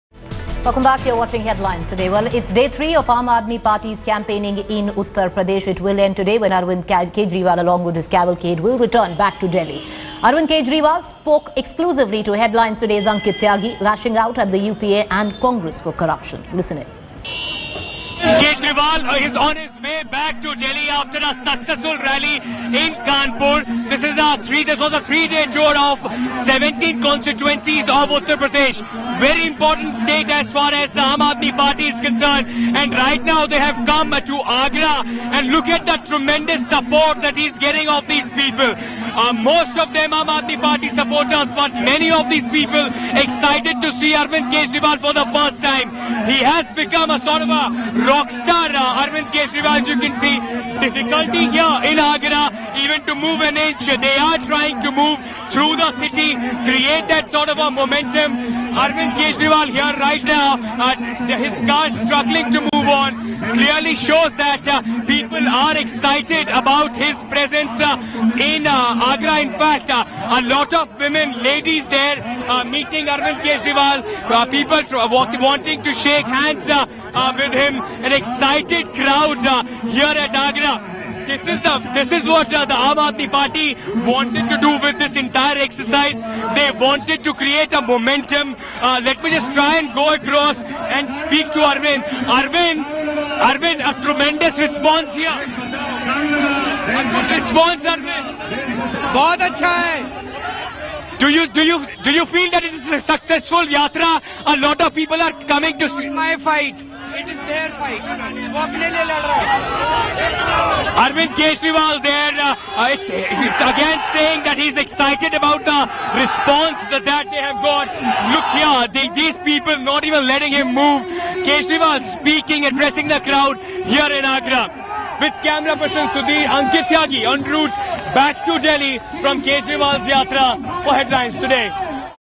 Kejriwal return to Delhi after rallying in UP
Aam Aadmi Party leader Arvind Kejriwal returns to Delhi via Agra after rallying in Uttar Pradesh for 3 days.